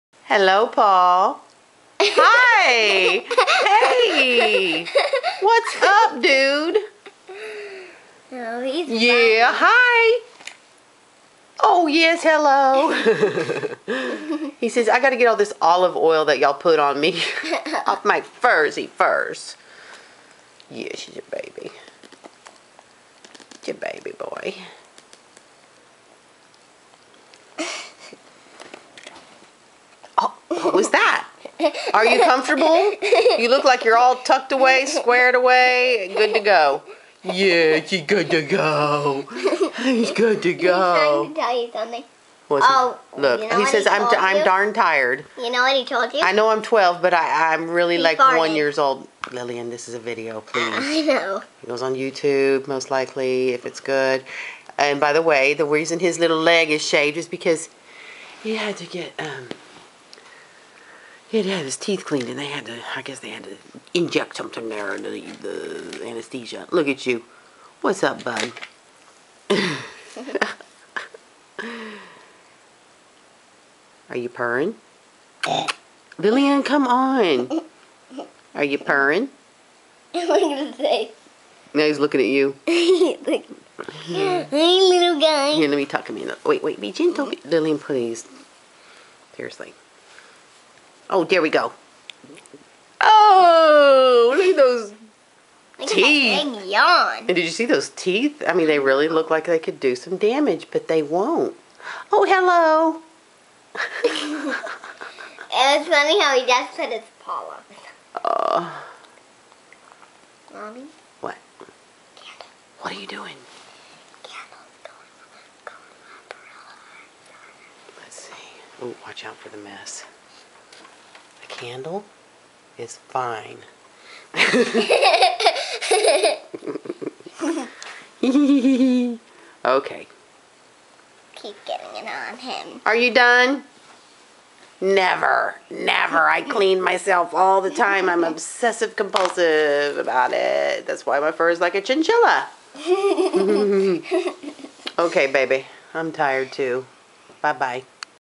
My cat Paul is soooo funny!! (Best video of him yet!)
Paul waving to me :O)